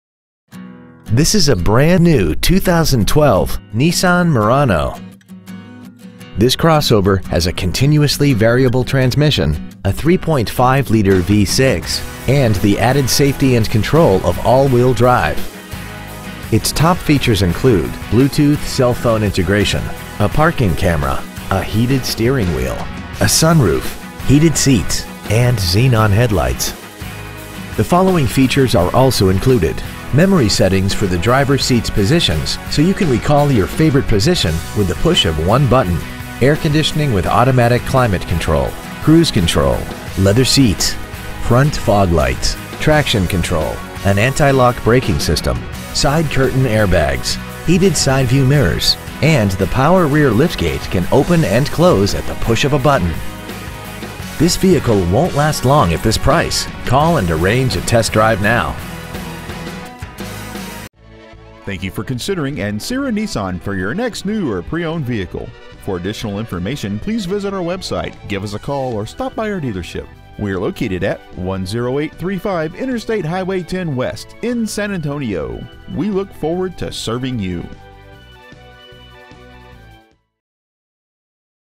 2012 Nissan Murano San Antonio TX
Year : 2012
 Make : Nissan
 Model : Murano
 Engine : 3.5L V6
 Trans . : Continuously Variable (CVT)
 Exterior : Super Black

 Interior : Char
 Stock : 222421